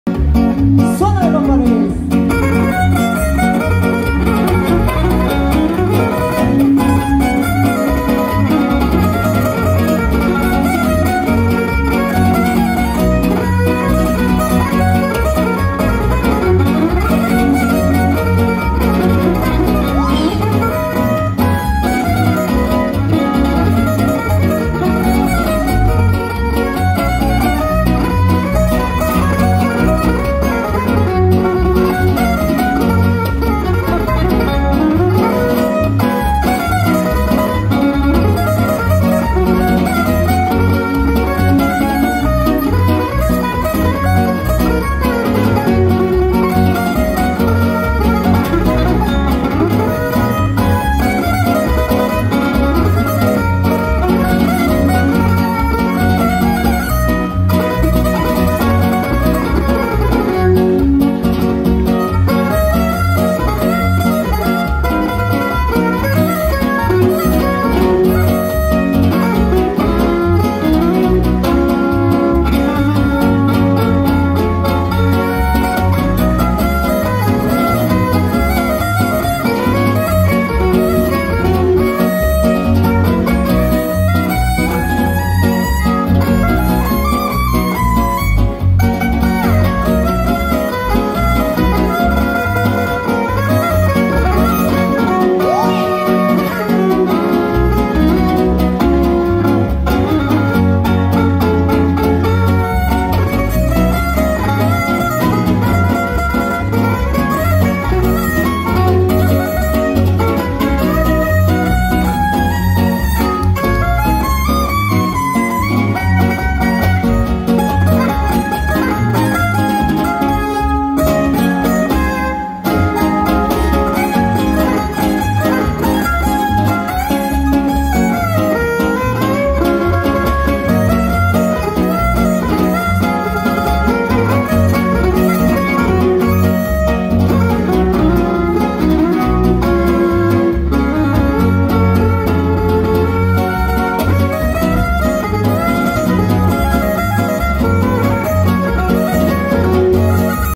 En vivo desde Raleigh NC Trio La Nueva Era Polka
La Última Huapangueada de la decada en Raleigh NC Trio La Nueva Era. Informes y contrataciones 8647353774